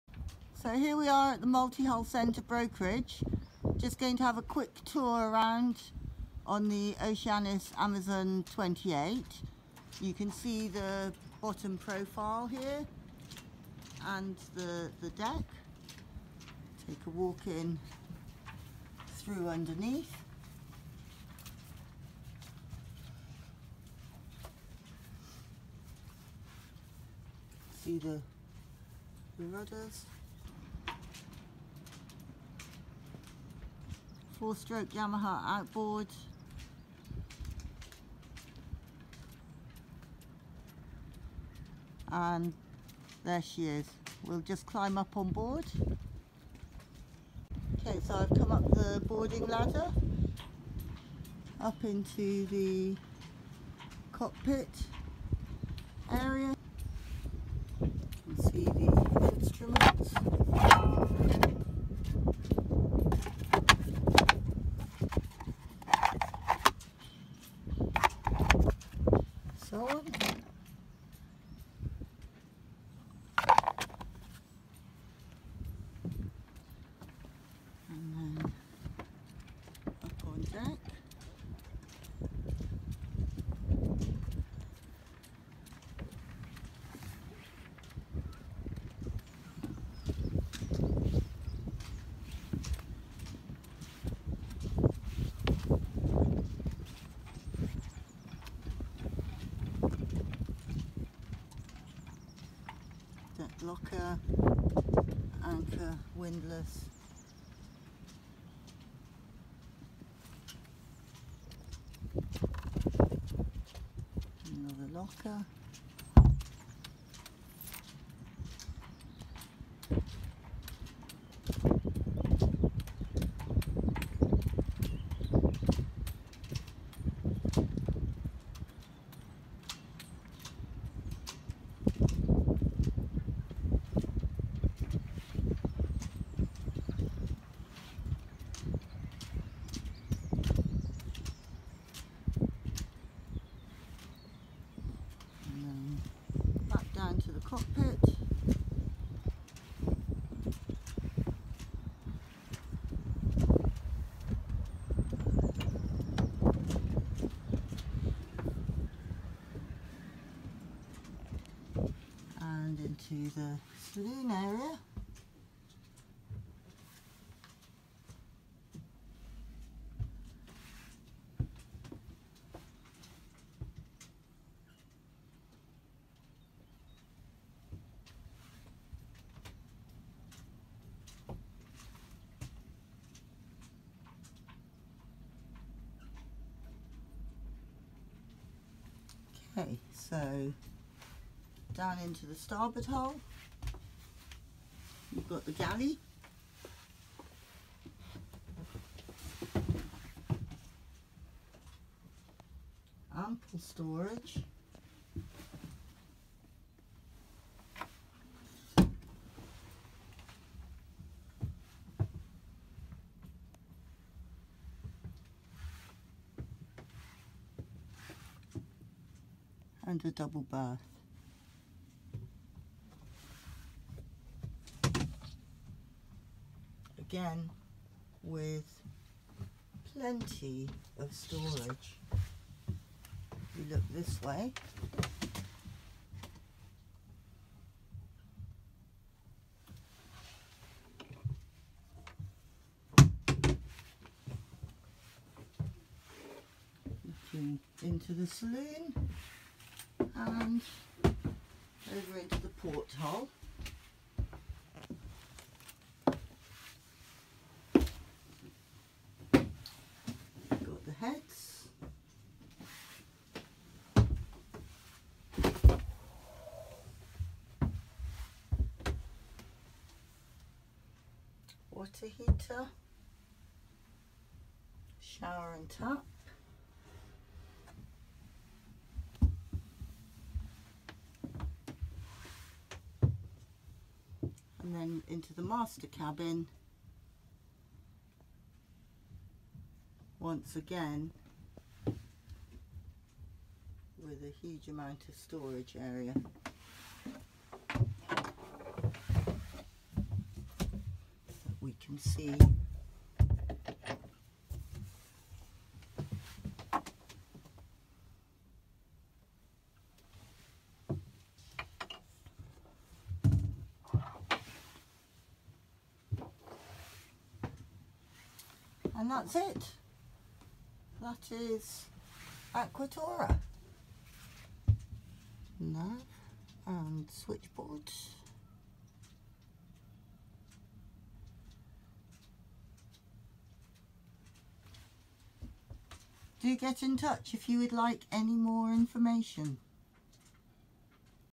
Oceanic Amazon 28 for sale
SOLD
Lying near Plymouth UK. At just £17,000 this cat makes a great little getaway, all ready to go.
Find out more on our brokerage page, full specs, photos & 360 images